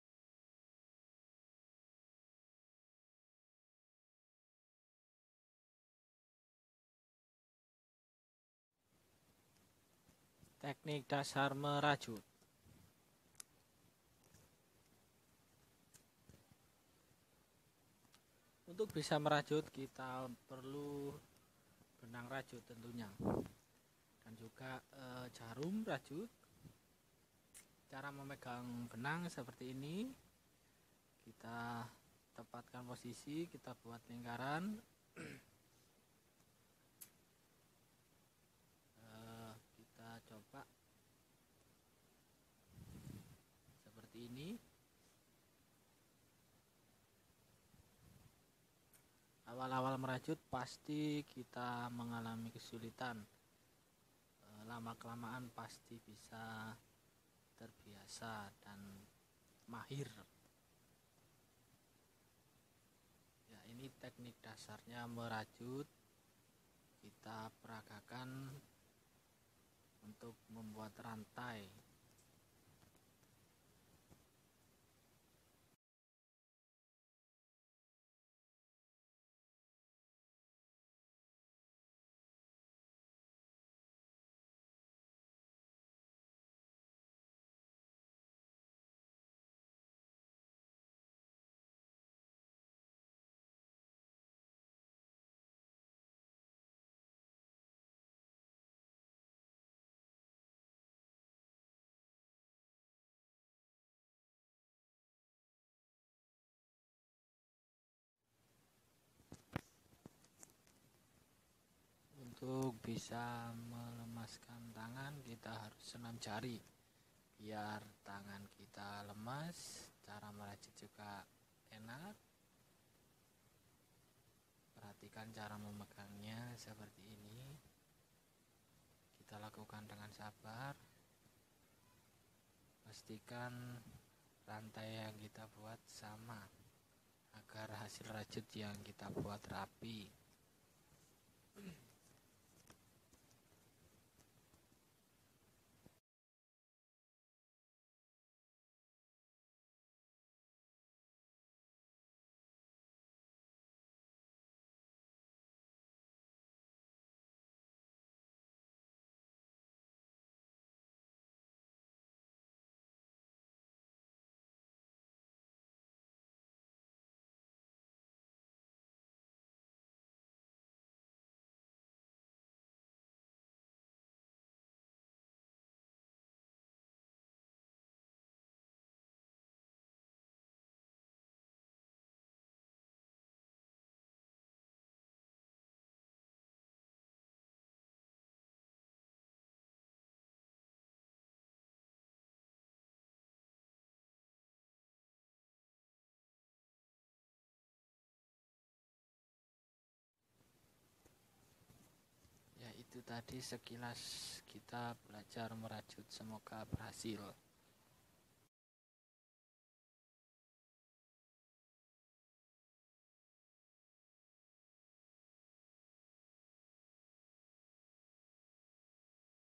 merajut untuk pemula
Rajutan adalah hobi dan kini sudah menjadi trend anak muda.

Merajut perlu keahlian dasar ilmu kesabaran dan ketekunan
Nantikan video terbaru kami